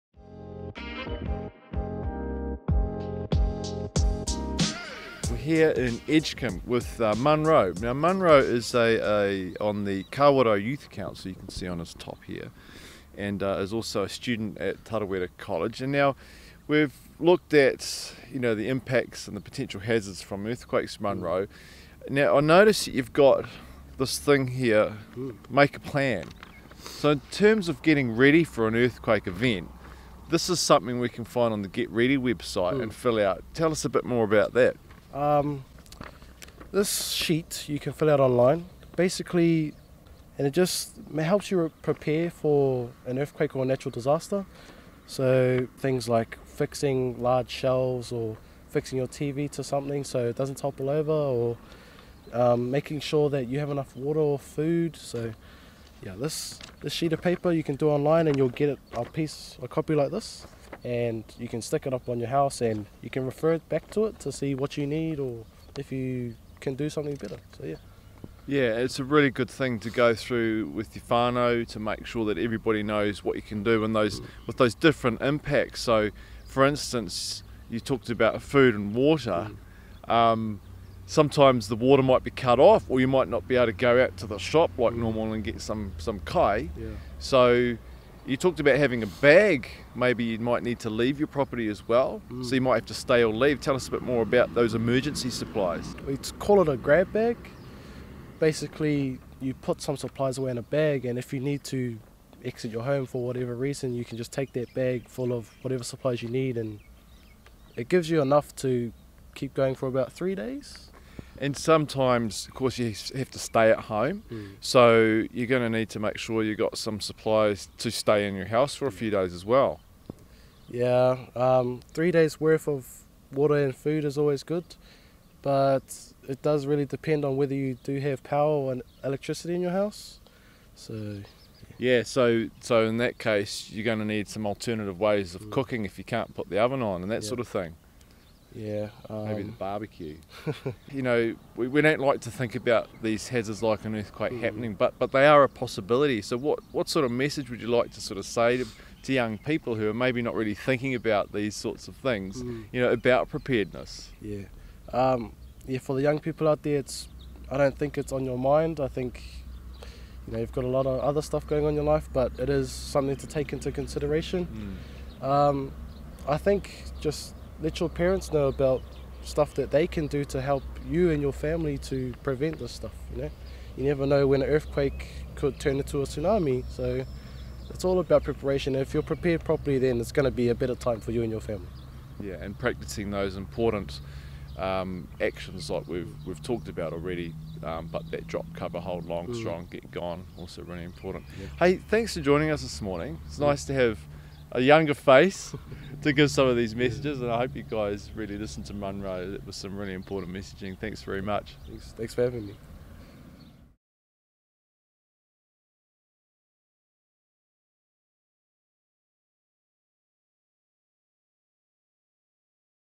Plan and Get Ready – LEARNZ What's the Plan, Stan? online field trip
We know about Drop Cover Hold to help prevent injury in an earthquake. But what other actions can we take to prepare for the impacts of an earthquake?